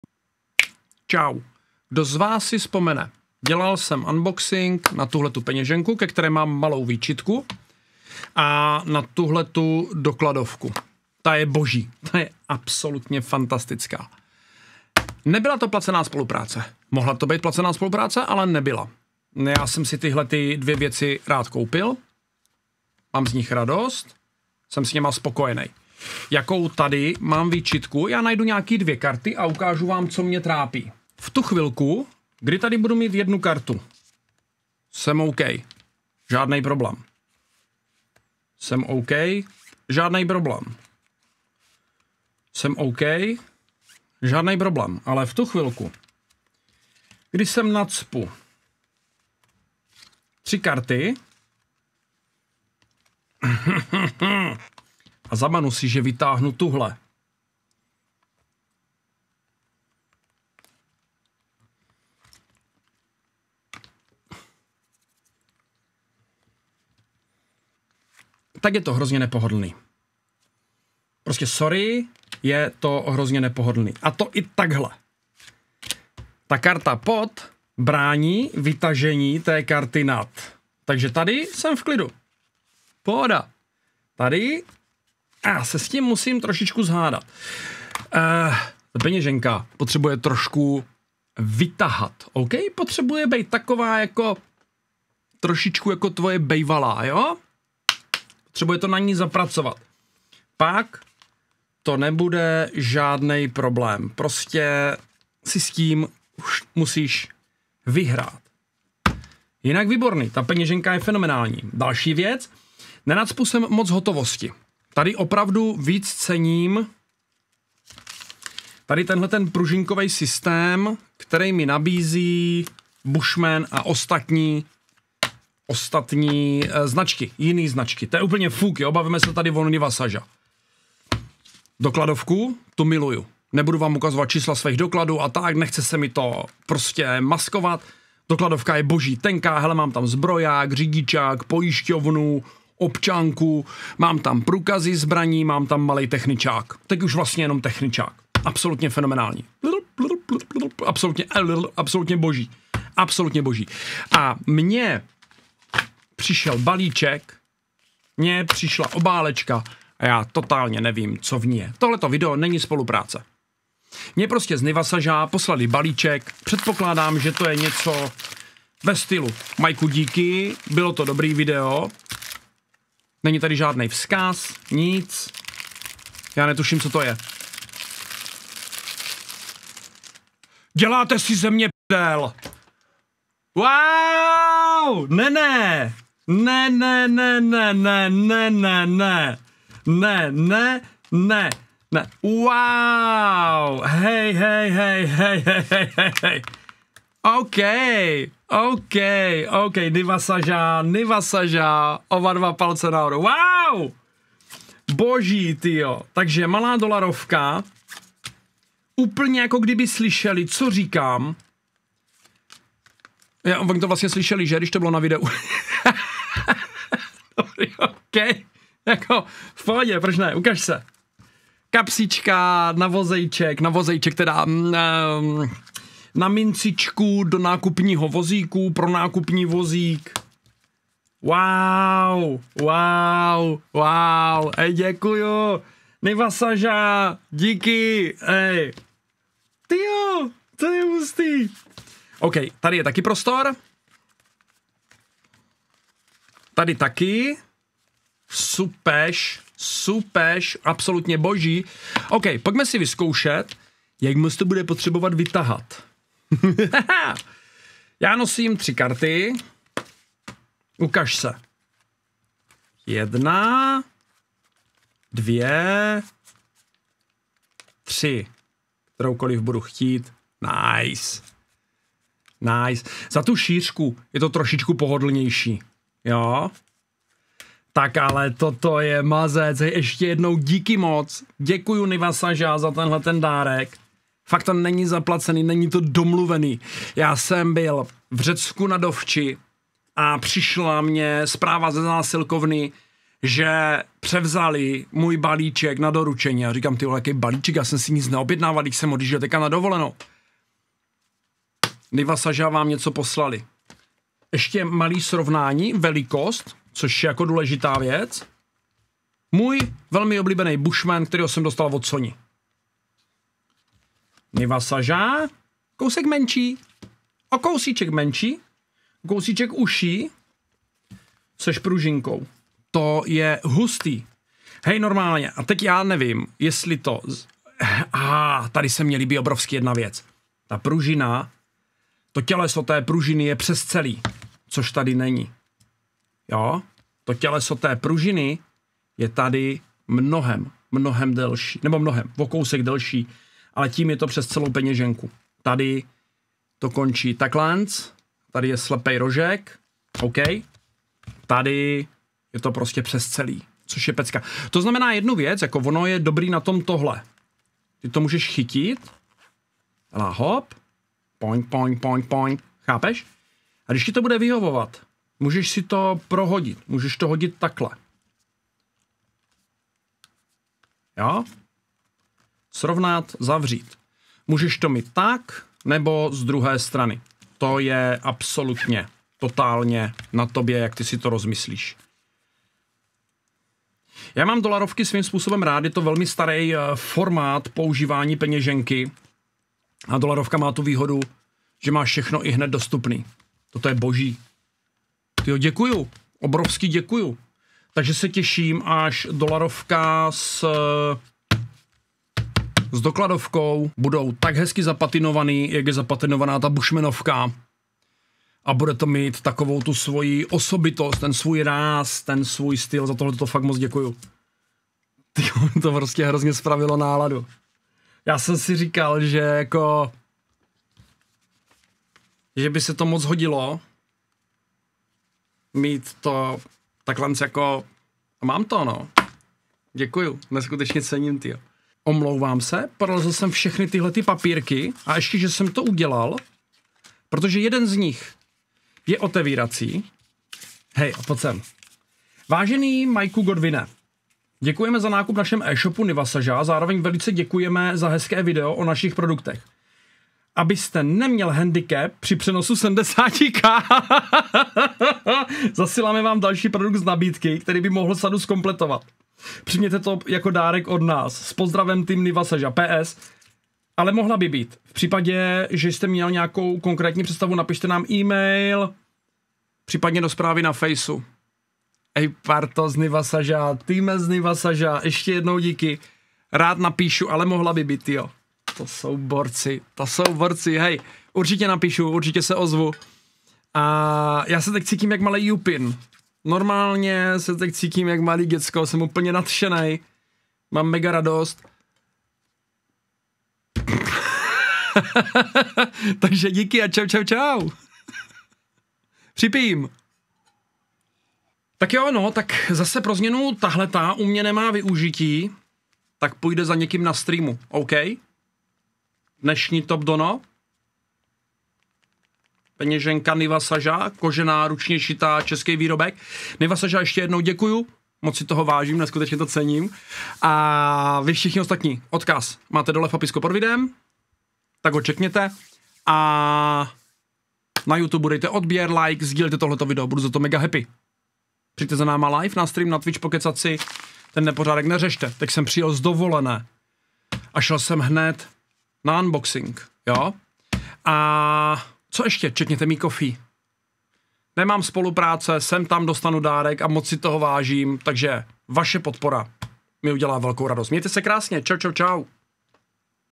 Překvapení od Nivasaža...
Tohle bylo ultra překvapení s parádním vzkazem. Moc @NivasazaCz děkuji a určitě se ozvu. Tohle video opravdu není placená spolupráce a já si tohoto dárku moc vážím.